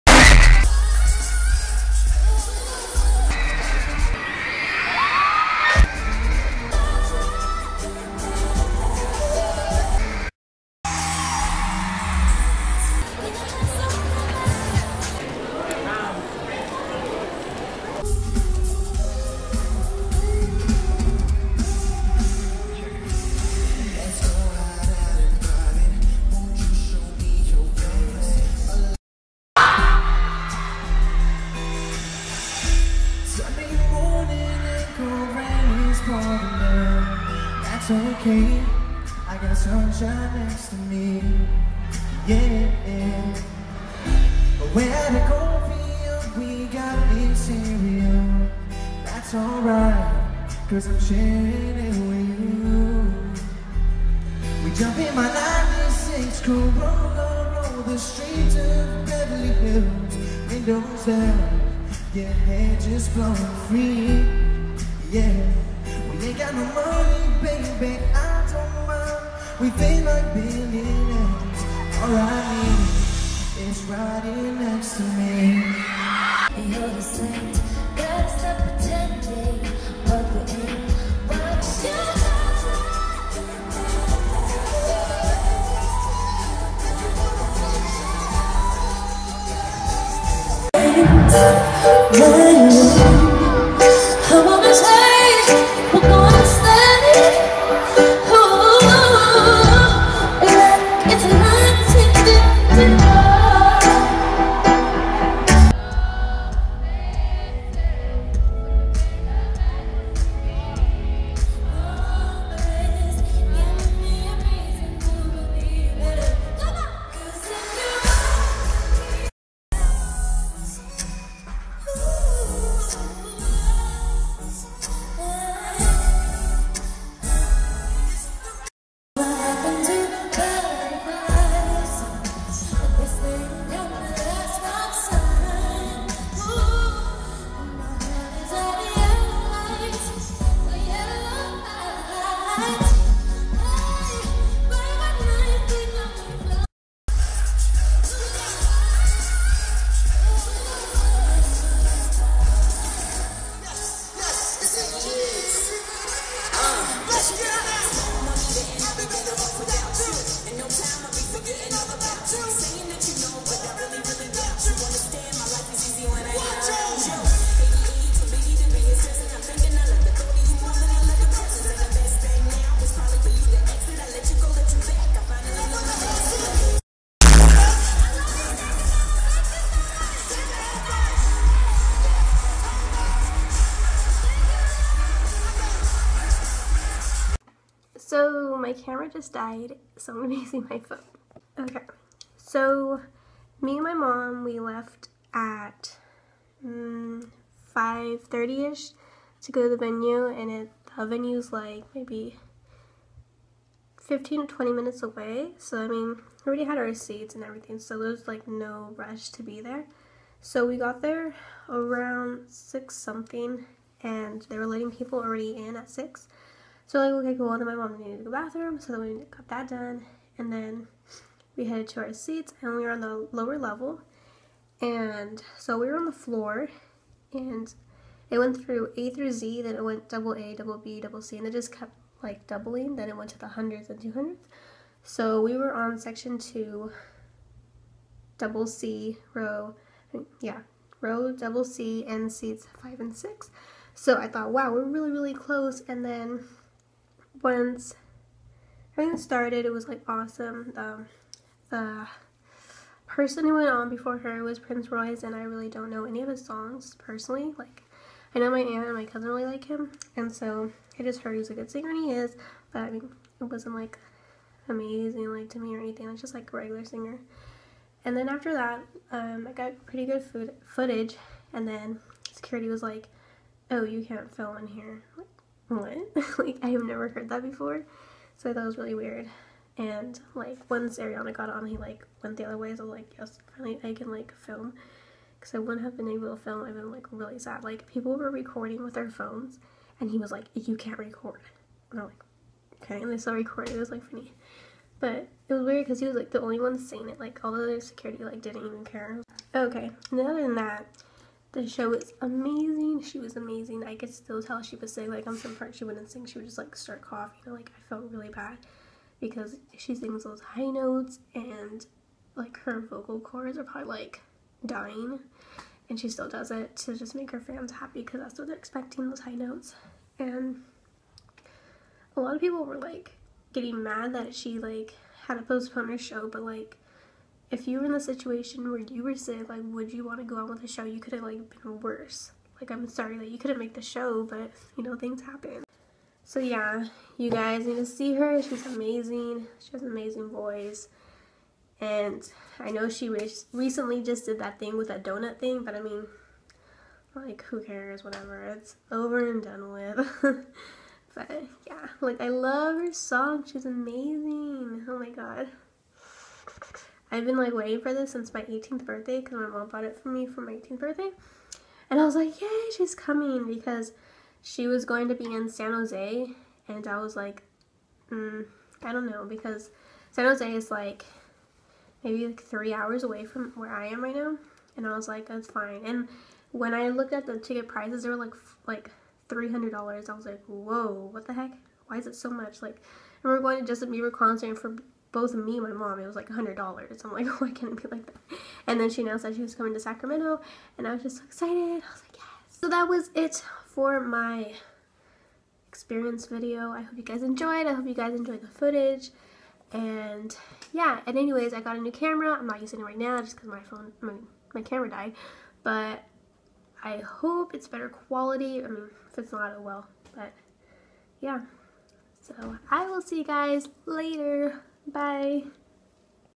Ariana Grande HoneyMoon Tour Experience 9-10-15
Hey guys!
Yesterday I went to the Ariana Grande concert in Sacramento ca & it was amazing!